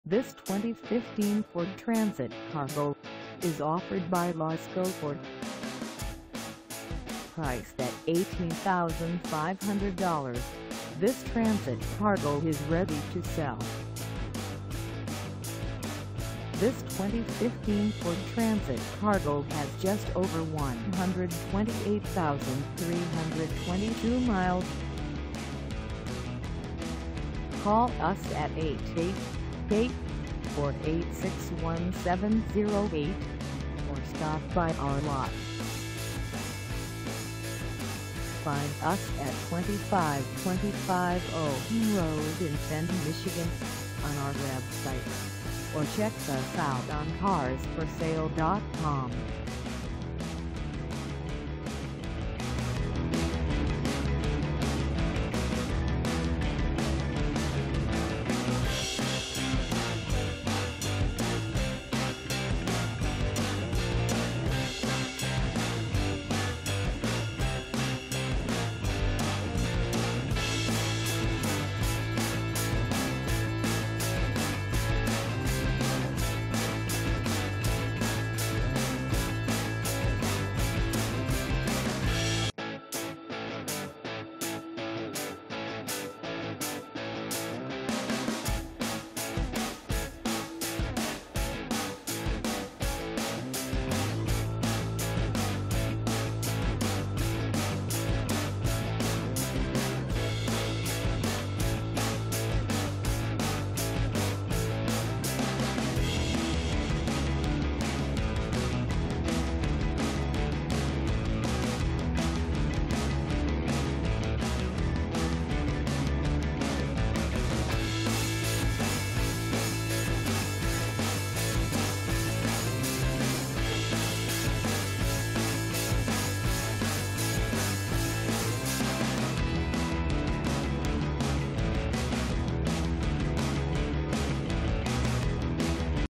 2015 Ford Transit Cargo 250 3dr LWB Medium Roof Cargo Van w/
This 2015 Ford Transit Cargo 250 3dr LWB Medium Roof Cargo Van w/Sliding Passen is for sale in FENTON, MI 48430 at LASCO FORD.

Engine: 3.7L V6
Transmission: Automatic 6-Speed
Color: White
Mileage: 128322
Contact LASCO FORD 2525 OWEN RD FENTON, MI 48430 to test drive this 2015 Ford Transit Cargo 250 3dr LWB Medium Roof Cargo Van w/Sliding Passen today.